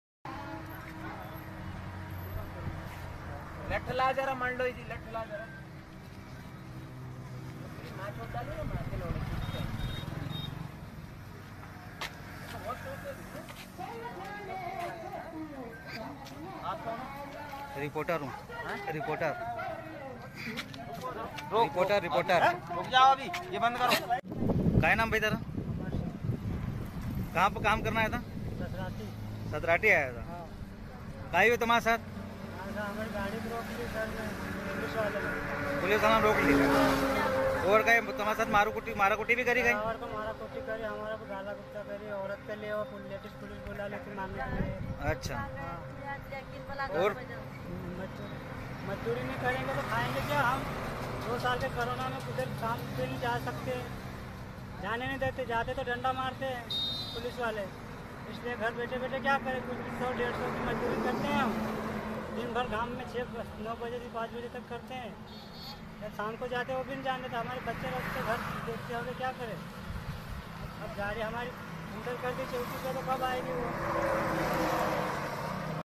एक तो गरीब ऊपर से सेना के जवान की लात, उफ़
"लठ लाओ ज़रा, मां ***"
MP के खरगोन के बलकवाड़ा PS इंचार्ज वरुण तिवारी ने बेबस मजूदर को लातों से कुचला, वीडियो बनाने पर पत्रकार से झड़प। TI लाइन अटैच।एक तो गरीबी, भुखमरी उपर से इस तरह की बर्बरता। उफ़...